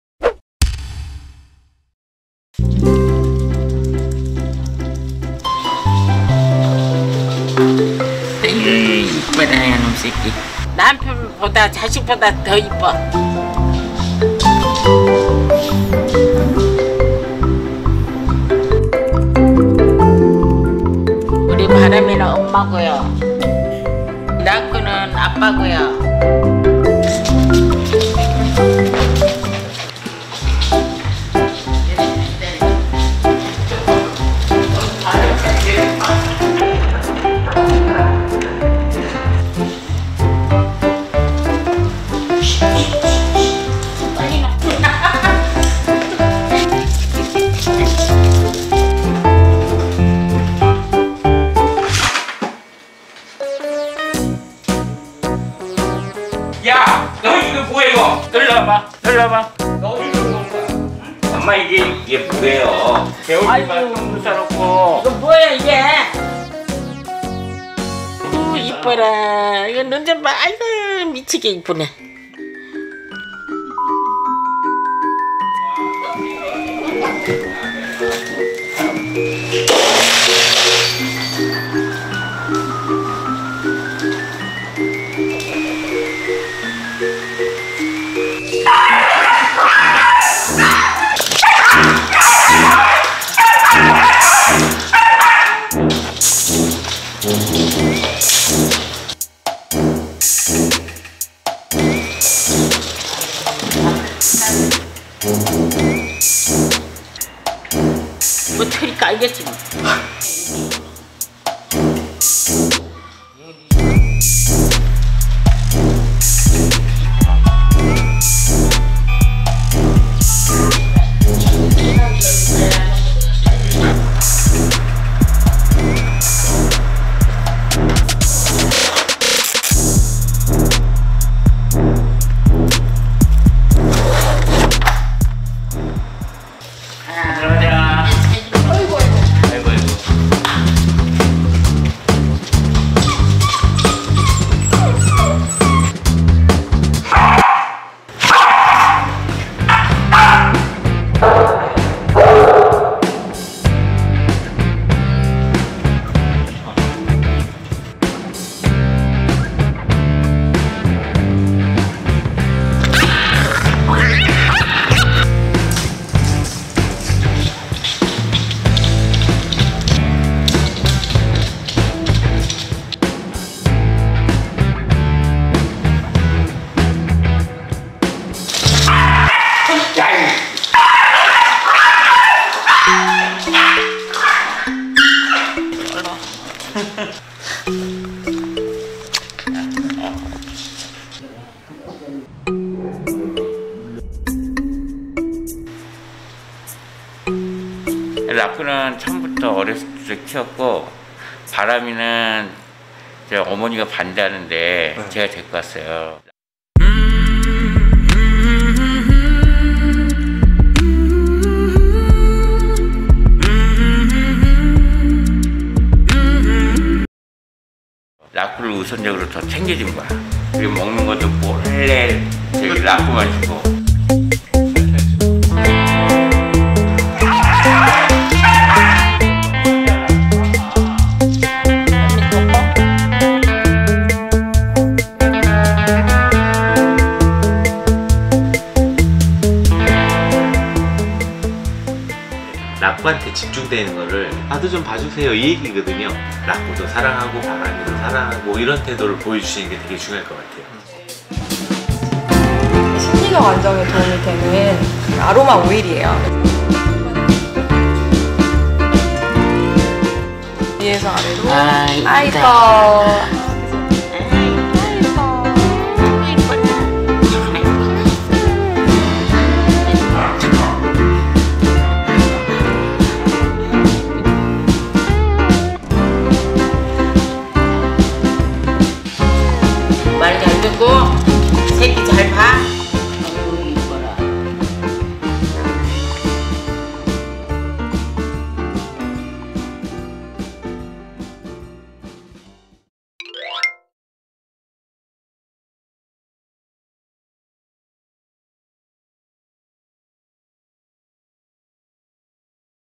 Grandma's Favoritism Drives Cutie Corgi Puppies Nuts?! | Kritter Klub
Dem cute corgi puppies grandma can't help but love the puppers even when they almost destroy the house! But the grandmother's favoritism actually triggered the puppers to go crazy..?